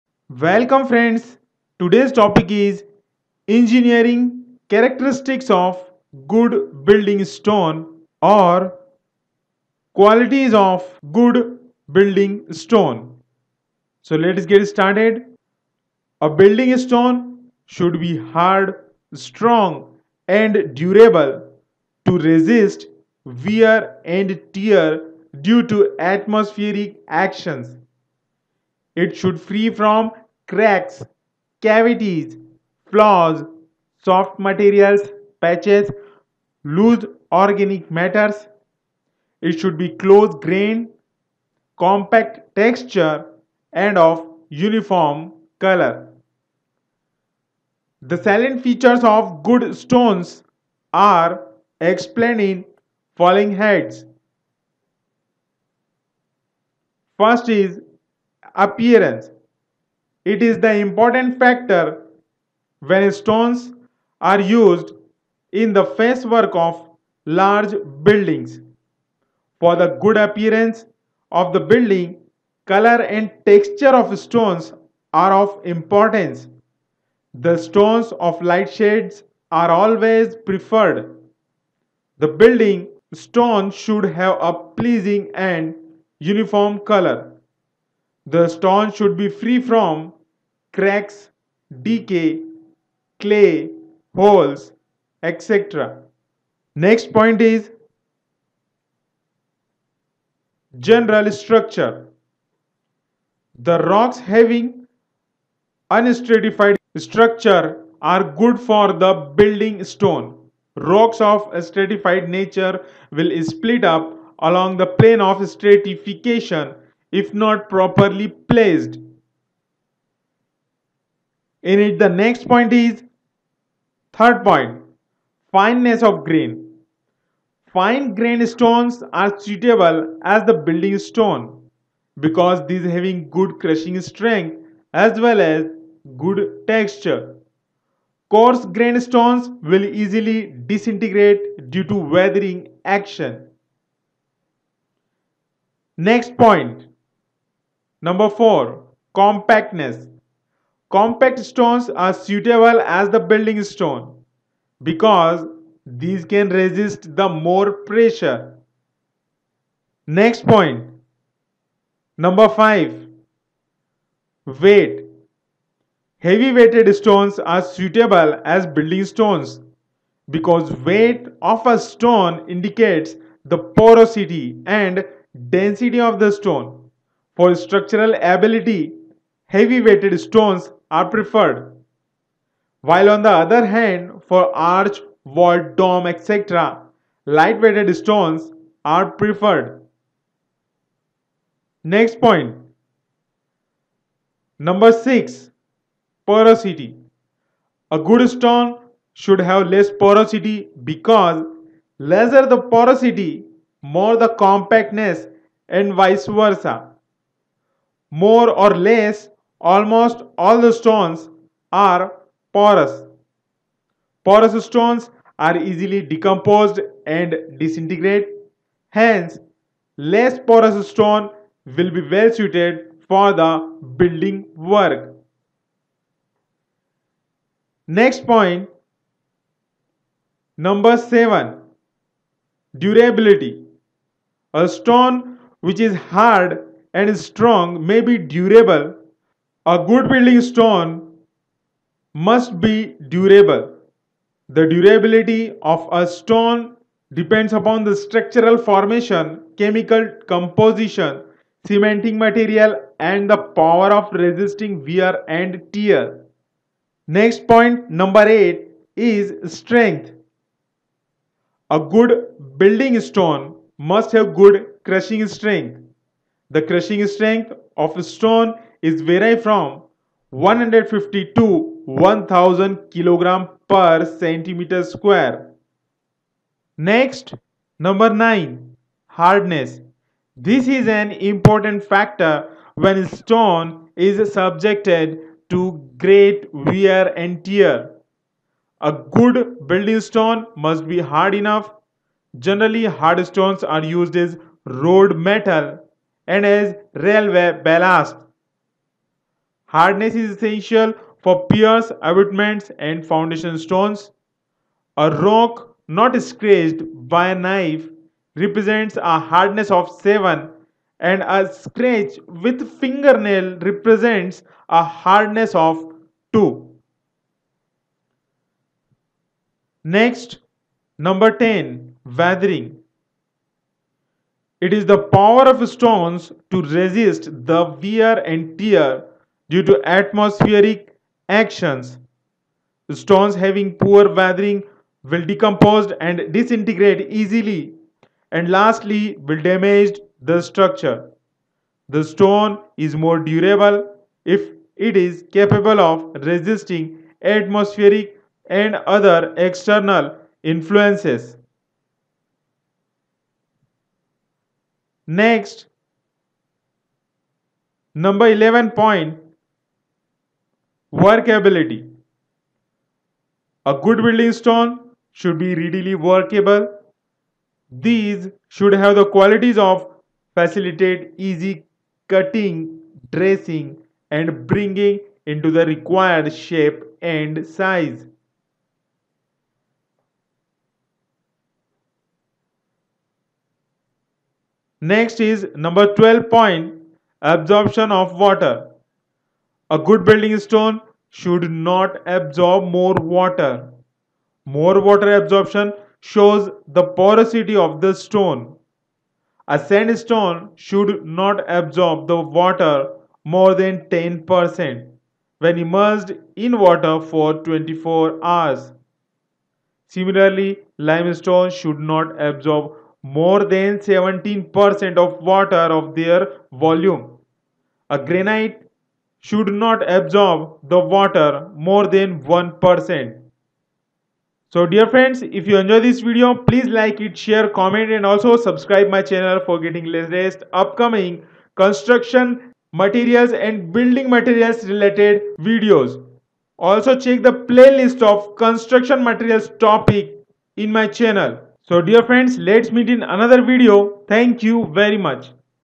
Engineering characteristics or qualities of good building stone, Construction and Building materials
A building stone should be hard, strong and durable to resist wear and tear due to atmospheric actions. It should free from cracks, cavities, flaws, soft materials patches, loose organic matters. It should be close grained, compact texture and of uniform colour.
Qualities of good building stone :
1. Appearance
2. General Structure
3. Fineness of Grain
4. Compactness
5. Weight
6. Porosity
7. Durability
8. Strength
9. Hardness
10. Weathering
11. Workability
12. Absorption of Water

Rules:
1. You should like my video
4. Write comment in the comments section as “All done”
5. You should watch my full video